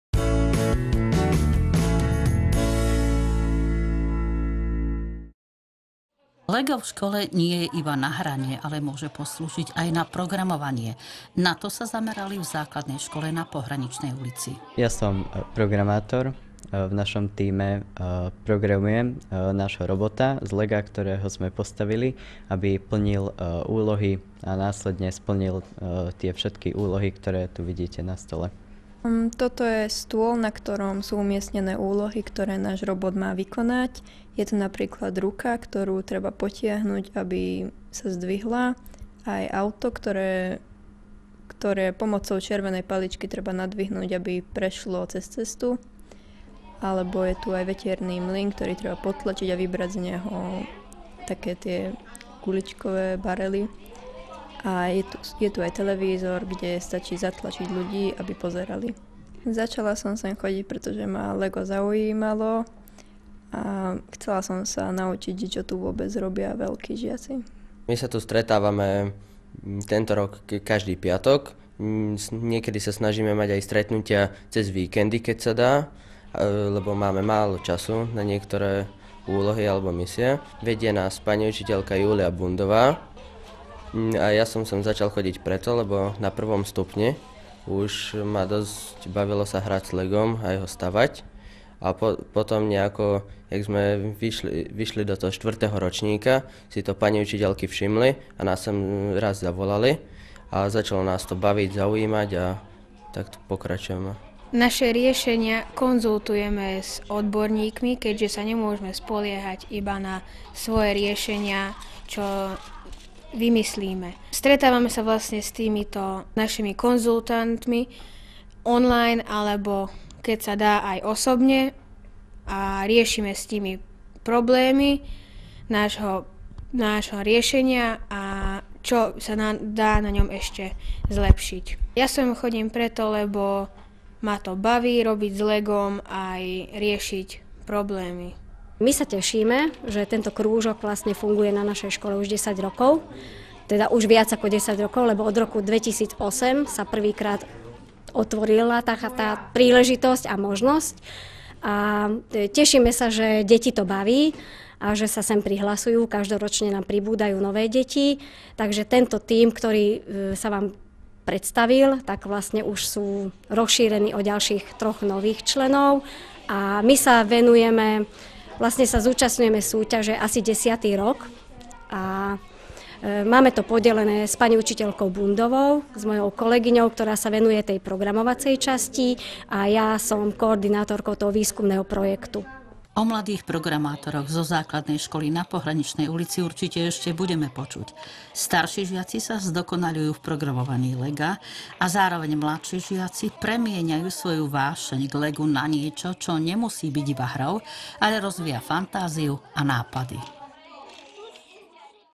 Rozvíja fantáziu a nápady
Lego v škole nie je iba na hranie, ale môže slúžiť aj na programovanie. Na to sa zamerali v Základnej škole na Pohraničnej ulici.